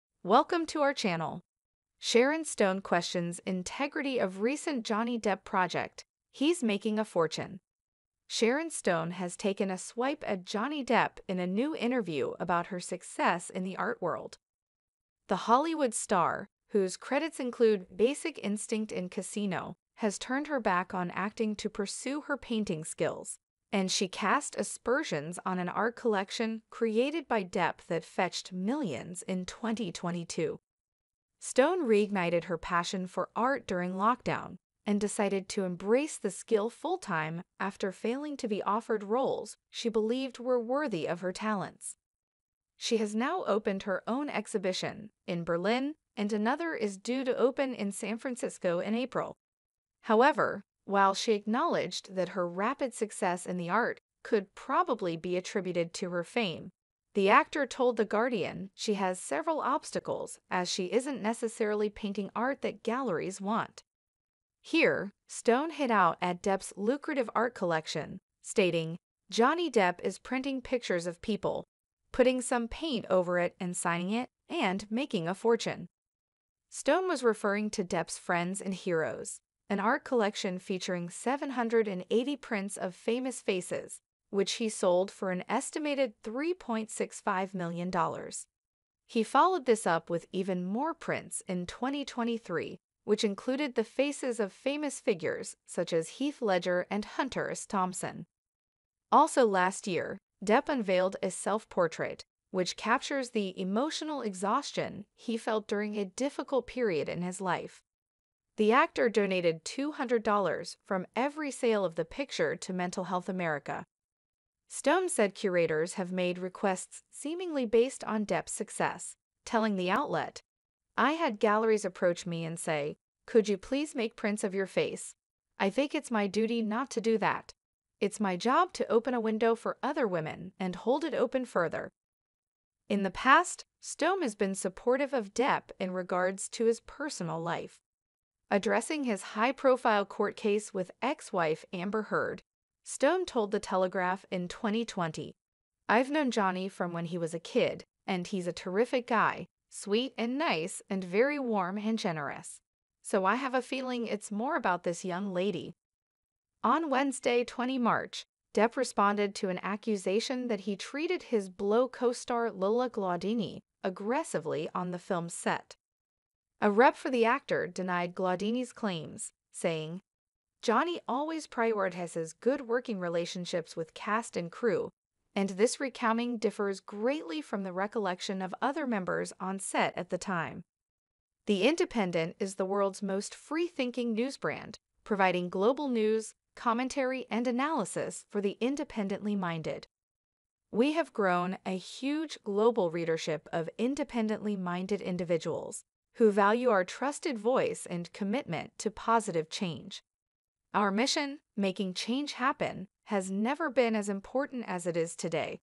Sharon Stone questions integrity of recent Johnny Depp project, ‘He’s making a fortune.
Sharon Stone has criticized Johnny Depp's recent art collection, Friends & Heroes, which sold for millions in 2022. Stone, who has rekindled her passion for art during lockdown, questioned the legitimacy of Depp's success. She argued that Depp is making a fortune by printing pictures of people, putting paint on them, and signing them. Stone also questioned Depp's treatment of his co-star Lola Glaudini on the set.